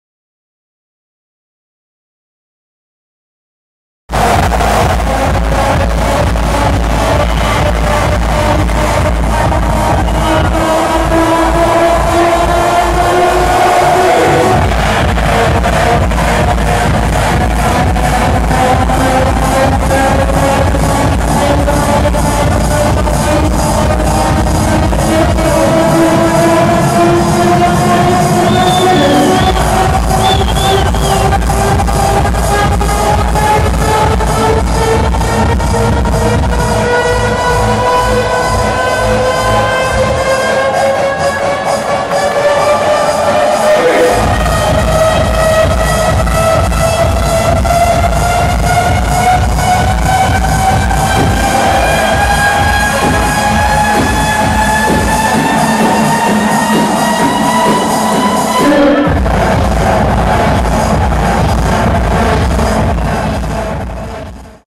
Carl Cox DJ Set - Space Ibiza (August 11st)
Space, August 11st. Carl Cox "Join Our Revolution" party at Space with French DJ-producter Tom POOKS. Real Techno, watch out ! This video show how intense Carl Cox can be ! Absolutely insane !